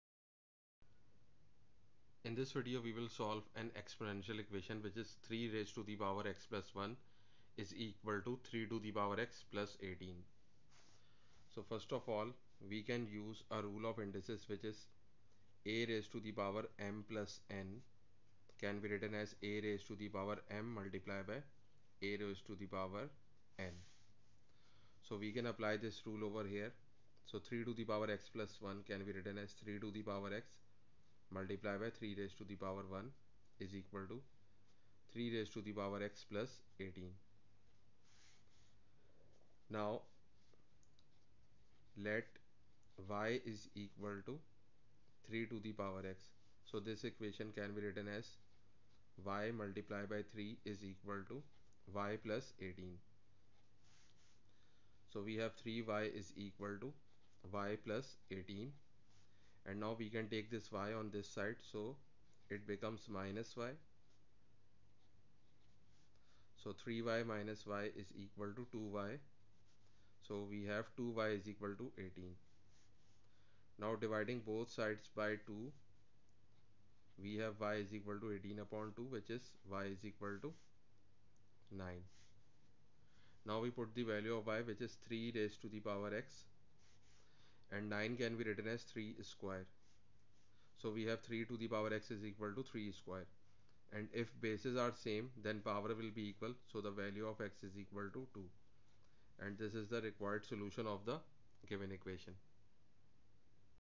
Exponential Equation || 3^(x+1)=3^x+18
In this video we shall learn how to solve an exponential equation.